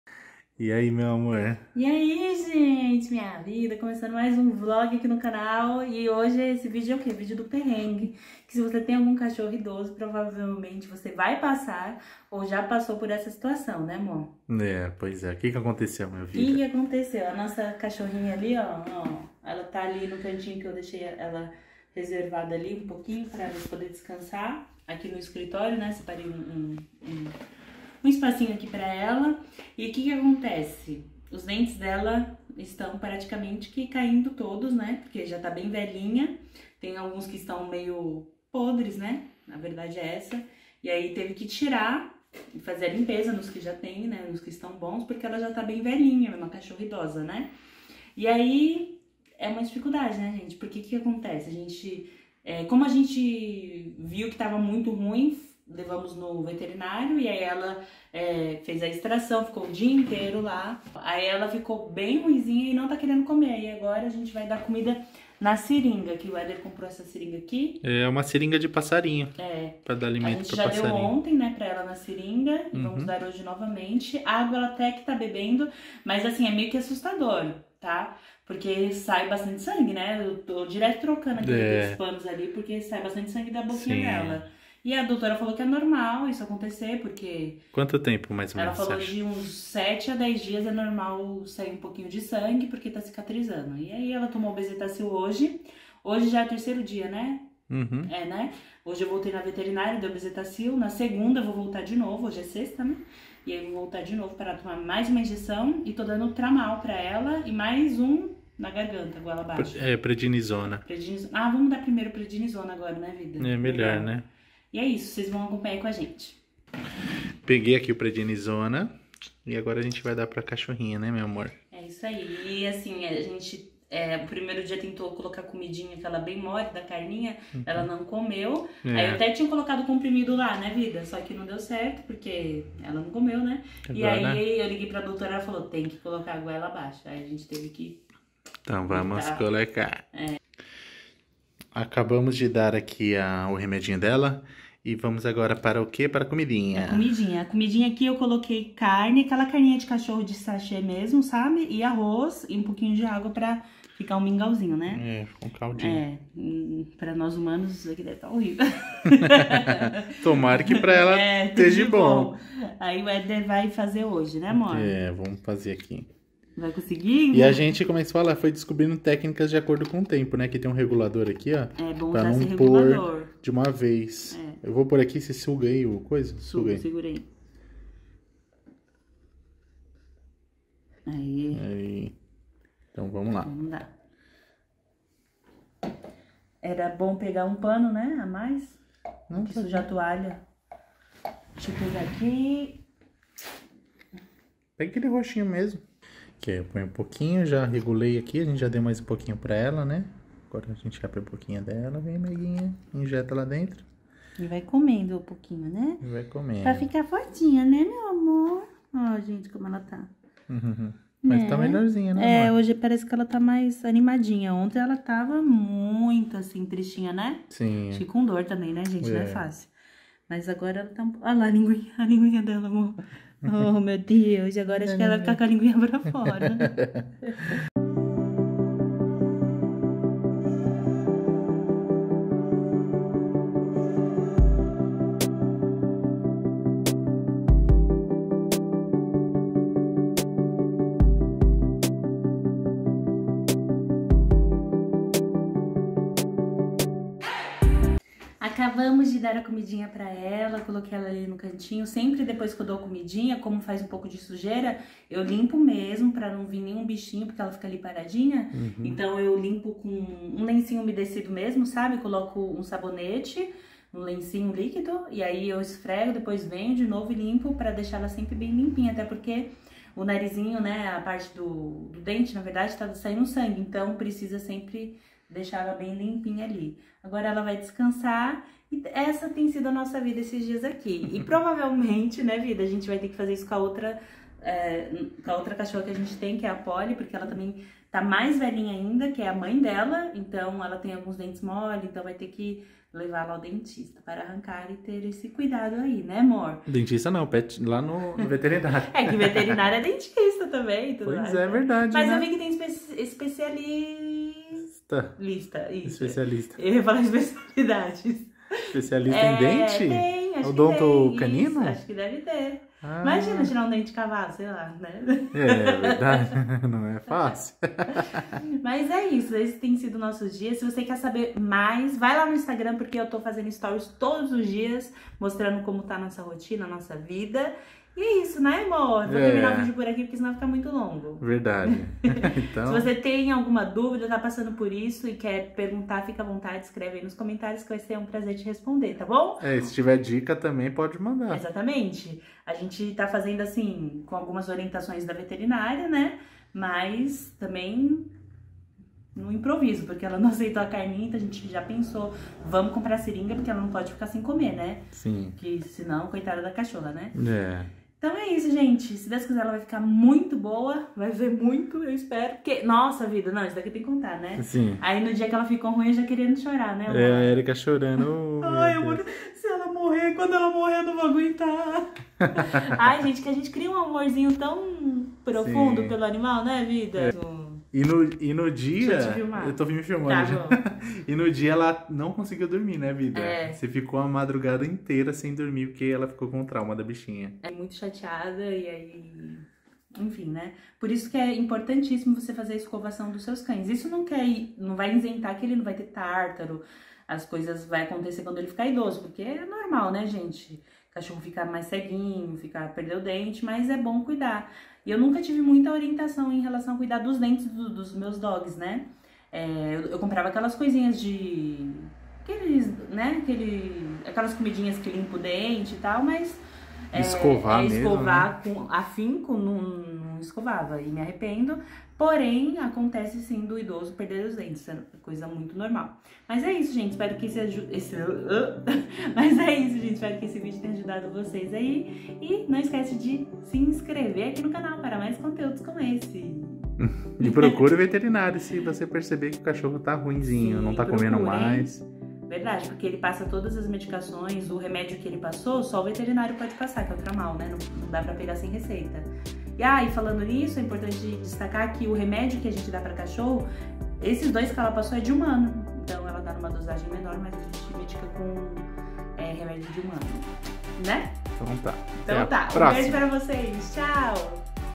MINHA CACHORRA IDOSA TIROU OS DENTES | COMO ELA VAI COMER? | VLOG
♥♥♥ ASSISTA TAMBÉM ESSES VÍDEOS ♥♥♥